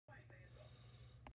Trip to Waffle House in Moss Bluff with my dad to see his coworkers
6:30am trip